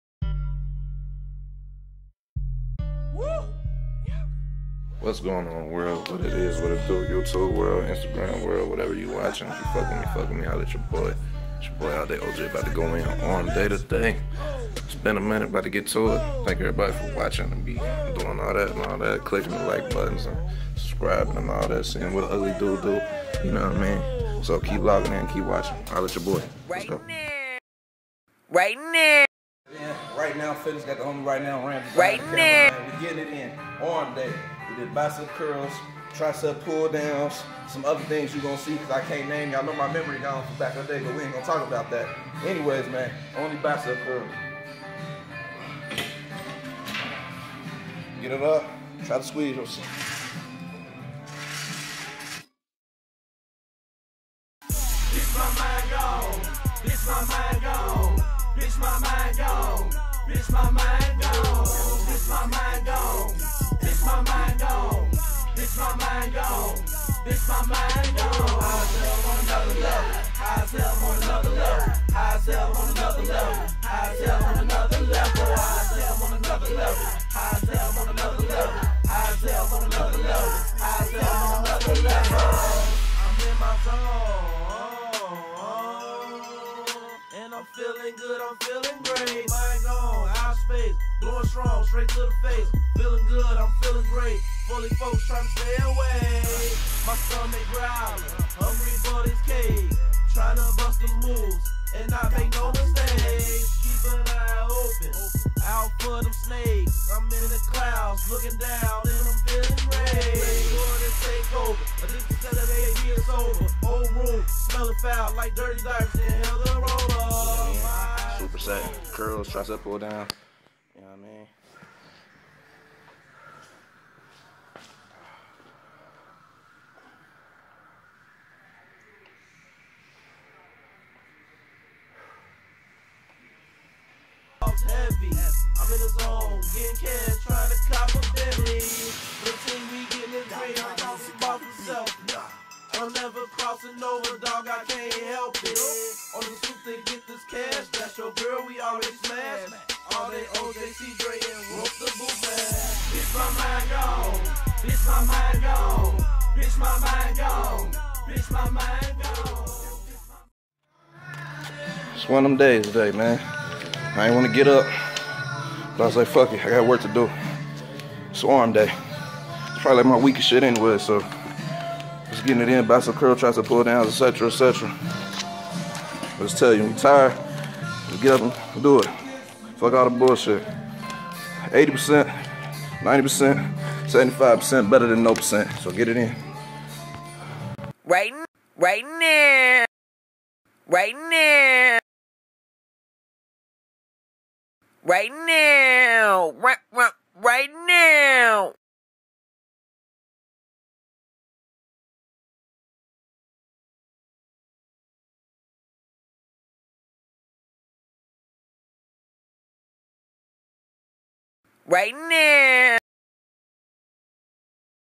All Day OJ On The Road to 4Hunnid episode 7
DescriptionDay 7 on the Road to 4oo. In this latest edition All Day Oj puts in work.
Ig alldayoj Fb Alldayoj twitter @alldayoj